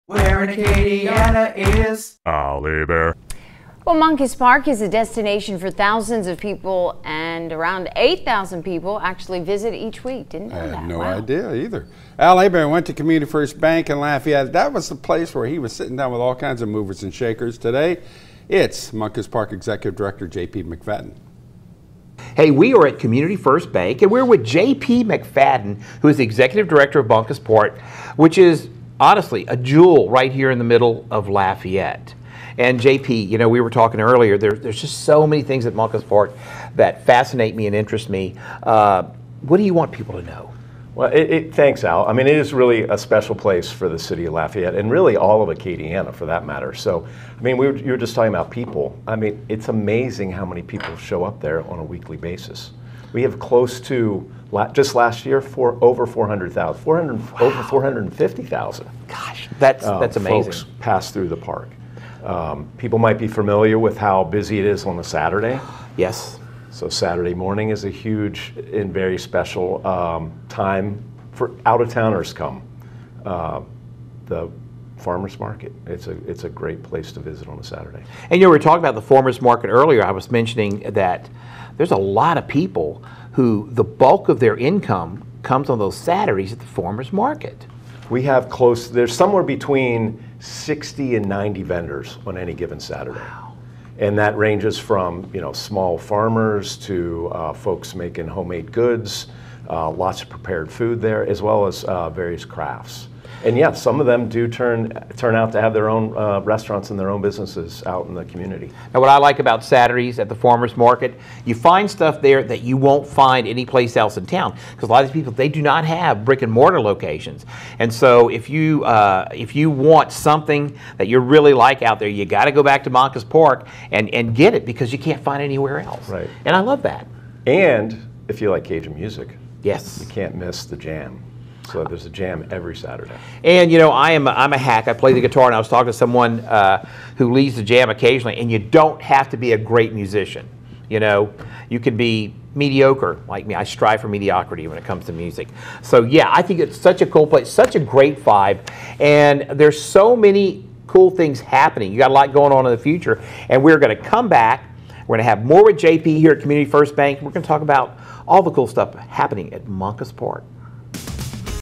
1-We sat down at Community First Bank of New Iberia in Lafayette to talk to J.P. MacFadyen,
Moncus Park